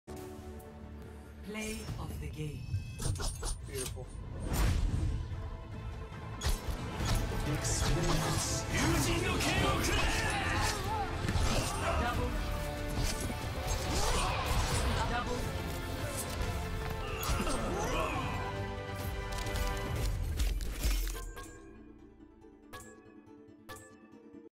81% Kill Participation
The Hanzo's 89% Match on Fire is actually more impressive, but he was a smurf with a Grandmaster main ... Still won ;)